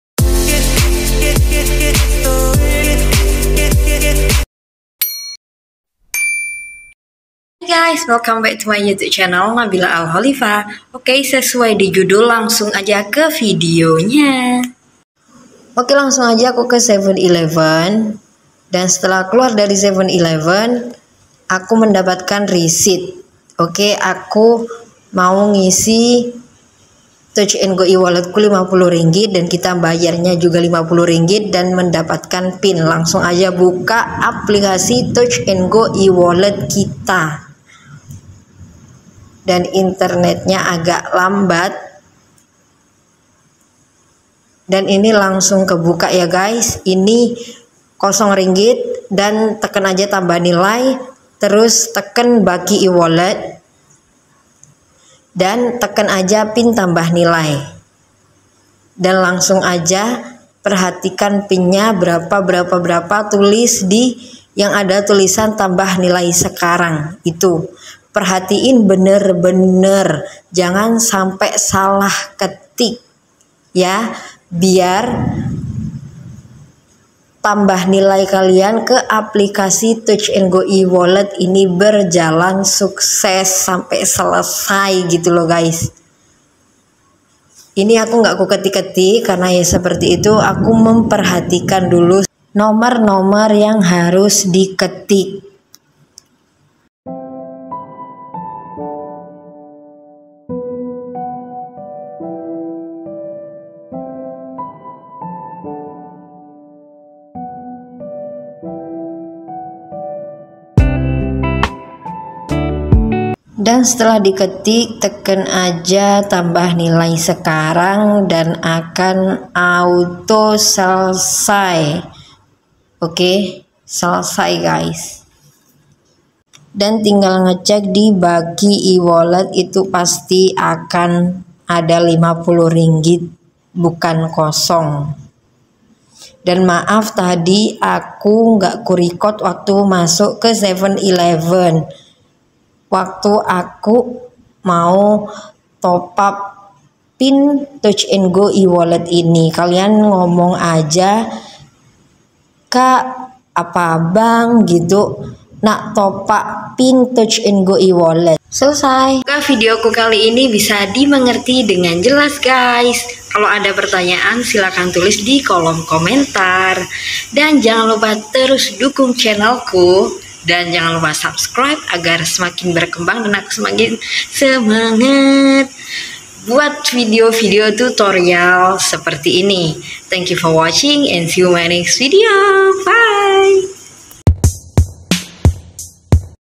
CARA ISI DUIT KE APLIKASI TOUCH N GO EWALLET MANGGUNAKAN DUIT CASH MELALUI 7ELEVEN?!?
Hi guys👋🏻
kalian pen tau cara isi TOUCH N GO EWALLET SECARA CASH MELALUI 7ELEVEN? Tonton video ini sampai habis,stay tune🎬

cara daftar aplikasi touch n go ewallet,
tekan ini👇🏻